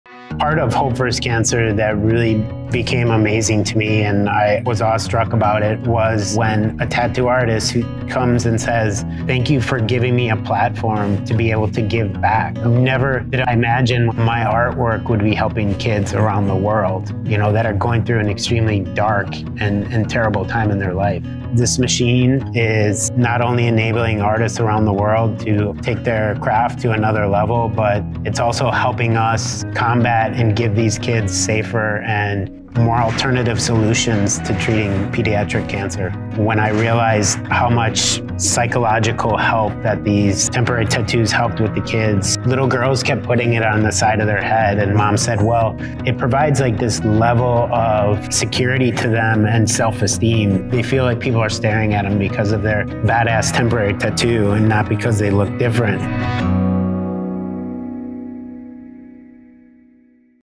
Joining Hope vs Cancer for Childhood Cancer Awareness
FK Irons is partnering with the Hope vs Cancer Foundation in the fight against childhood cancer. Every Golden Flux Max helps fund pediatric cancer research. Hope vs Cancer has a special connection to the tattoo community, using tattoo art to lift children's spirits and raise hope vs cancer.

This is the first, special edition Flux Max, inspired by the gold ribbon for Childhood Cancer Awareness. Join artists around the world in the fight against childhood cancer.

to join the fight.